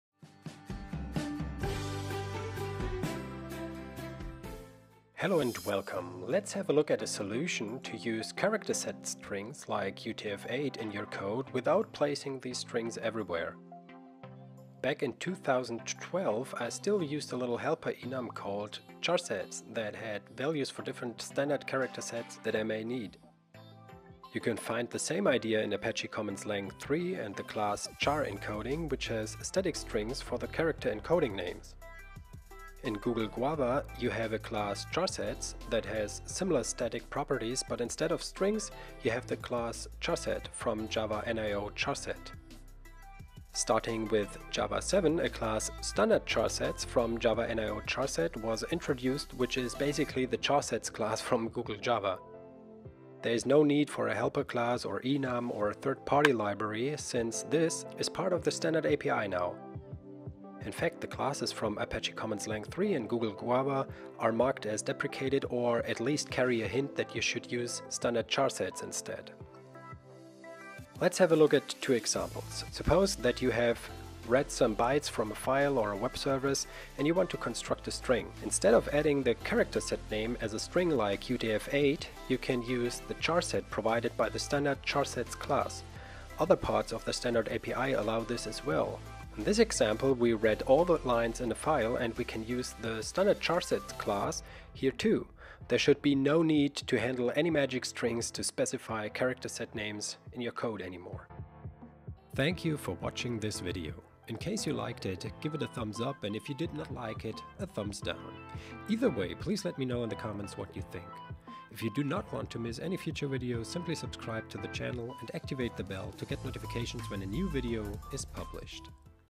Java: How to get UTF-8 charset constant | Advent calendar for developers
Let’s have a look at a solution to use character set strings like “UTF-8” in your code without placing these strings everywhere.